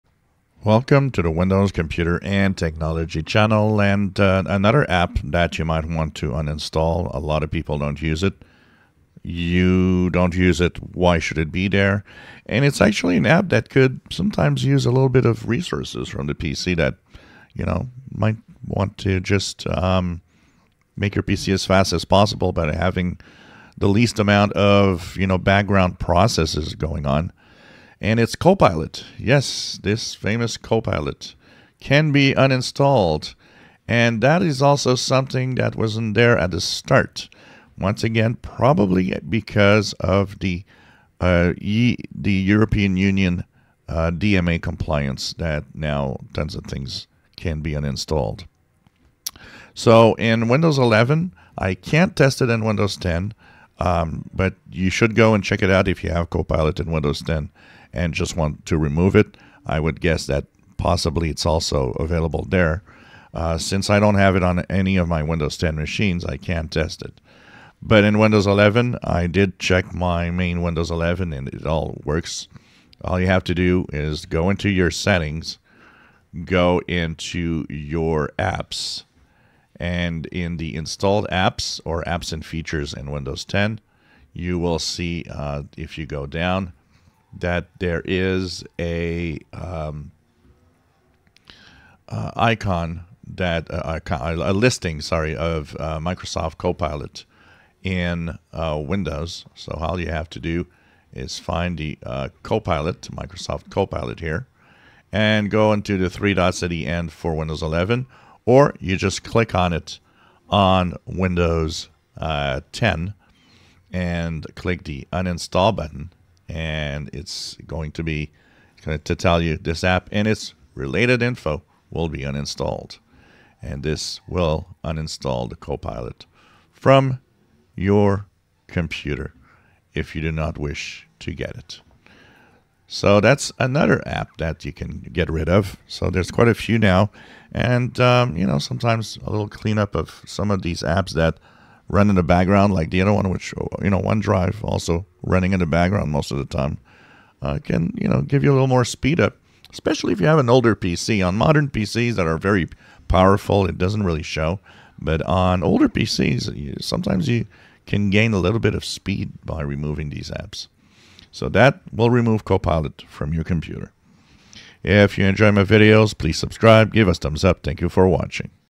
Windows 11 How to uninstall or remove Copilot from your PC
Copilot is another app that can be removed from your PC if you do not want it or use it